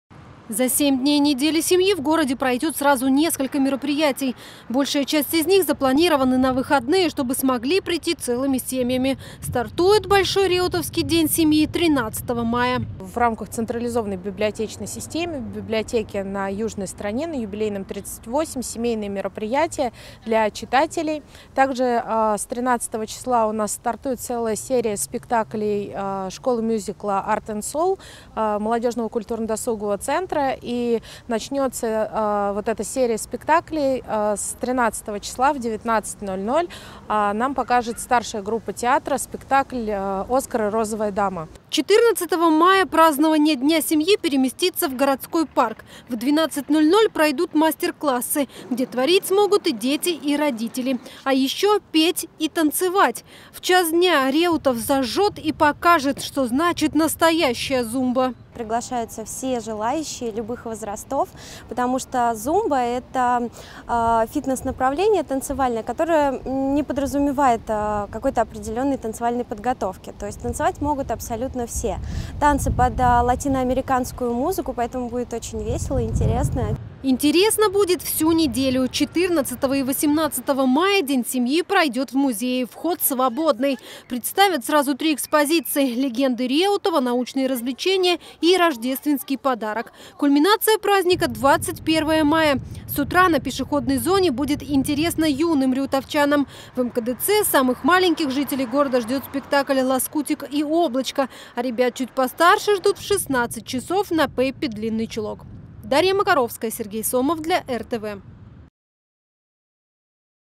«ДЕНЬ СЕМЬИ» В РЕУТОВЕ БУДУТ ОТМЕЧАТЬ НЕДЕЛЮ 12.05.2016
День семьи в Реутове решили отмечать всю неделю. Старт дадут уже - завтра - в пятницу. А основные мероприятия запланированы на субботу.